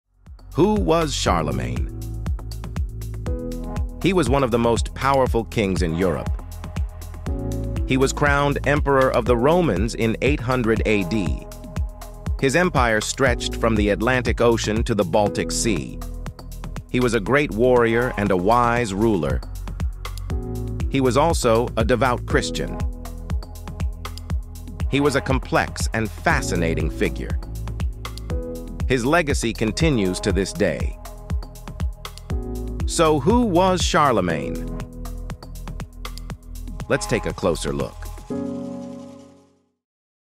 Who was Charlemagne? 🤔
👑 Step back in time as we explore the life and legacy of Charlemagne, one of the most influential figures in European history. Known as the "Father of Europe," Charlemagne's reign in the early Middle Ages left an indelible mark on the continent.

Join us to discover Charlemagne's history, with journey through the life of Charlemagne and the enduring impact of his rule on Europe. 🏰